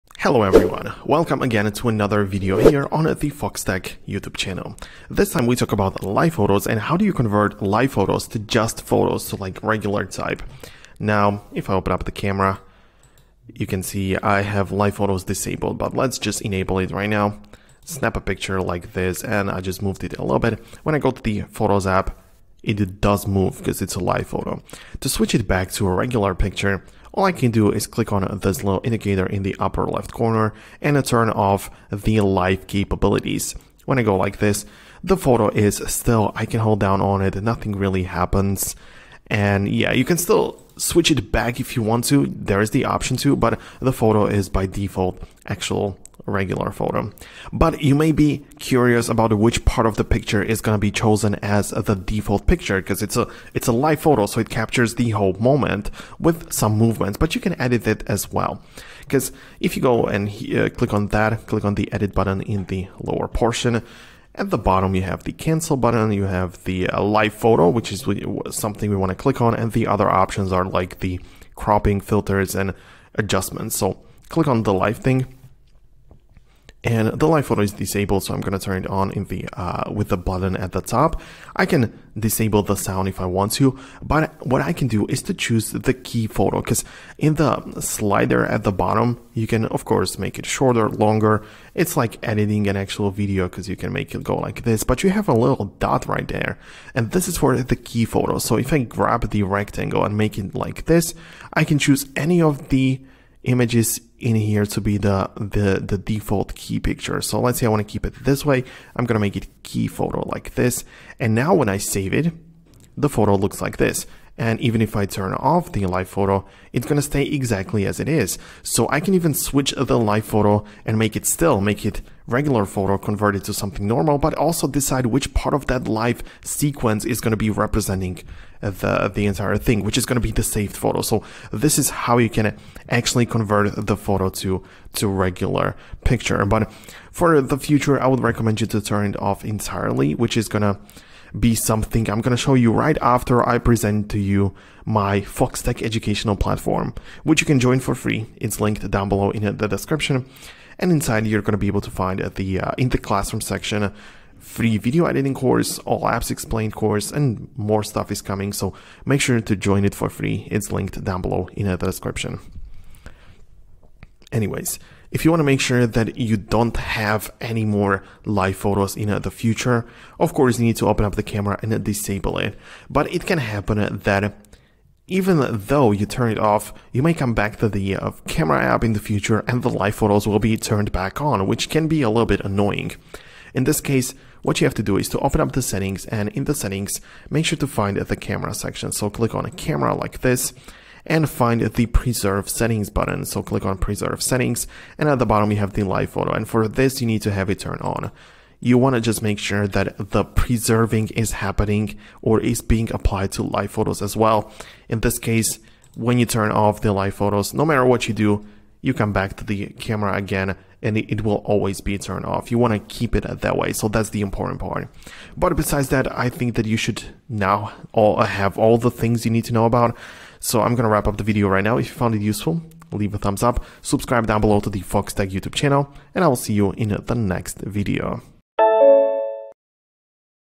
How do I Convert Live Photos to Just Photos on iPhone (tutorial)
Discover the quick and easy method to transform Live Photos into regular still images on your iPhone with our step-by-step tutorial. Whether you're looking to free up storage space or create a more traditional photo collection, we've got you covered. Join us to learn how to convert Live Photos effortlessly and streamline your iPhone's gallery!

Timestamps:
0:00 Introduction
0:21 Viewing Live Photos
0:48 Choosing the Key Photo
1:21 Disabling Live Sound
1:52 Conclusion